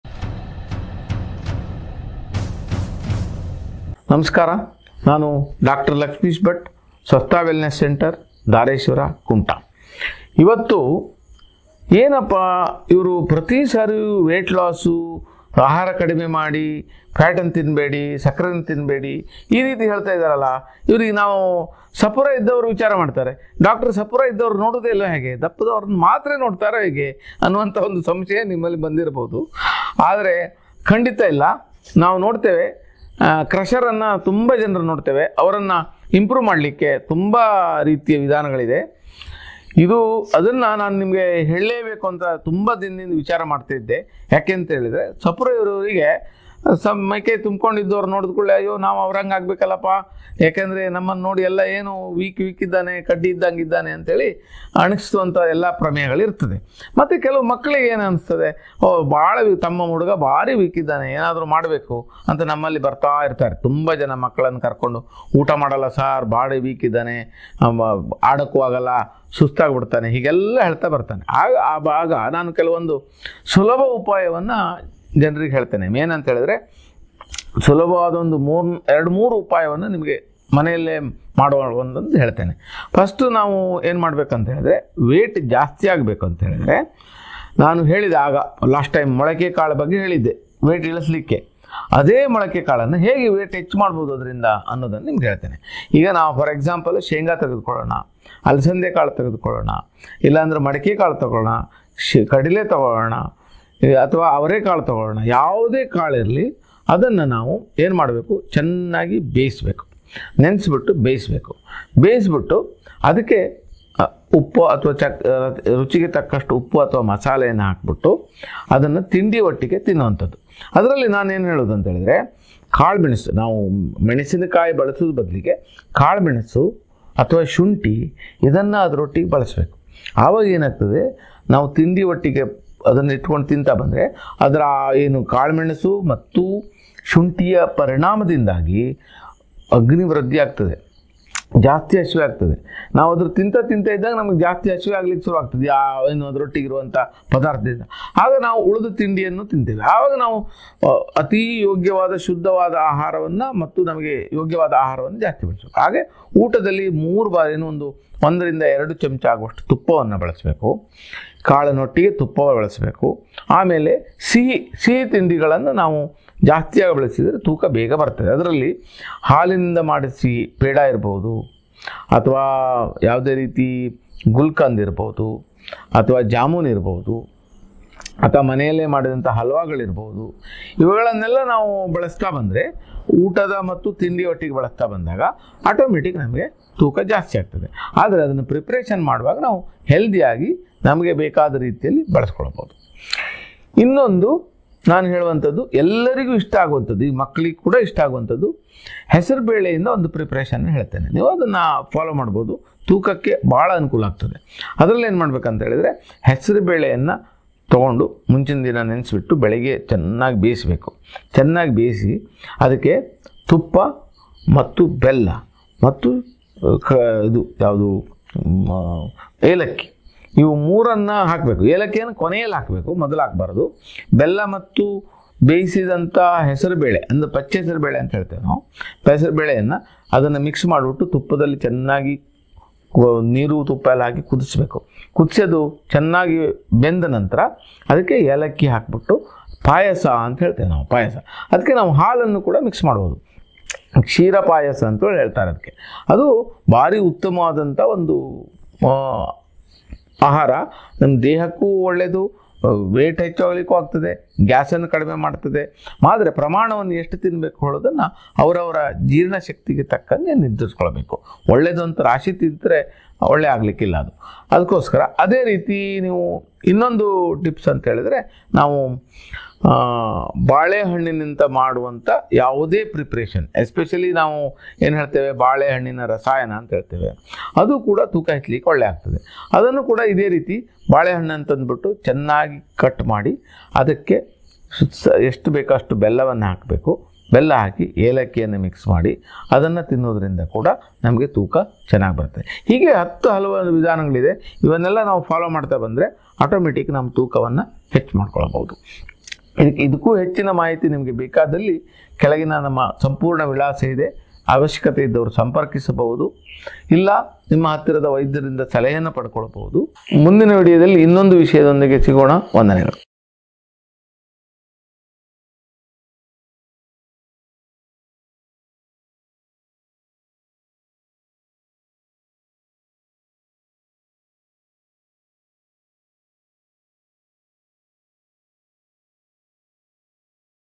ಈ ಕಾಳುಗಳಿಂದ ಪಾಯಸ ಮಾಡಿ ಕುಡಿದು ನೋಡಿ ನಿಮ್ಮ ತೂಕ ಹೆಚ್ಚಿಸಿಕೊಳ್ಳಿ Moong Dal Benefits
Swastha Ayurvedic Wellness Center
Dr.Laxmish Bhat B.A.M.S,FAGE
Near Janata Vidyalaya NH-66 Dhareshwar Kumata,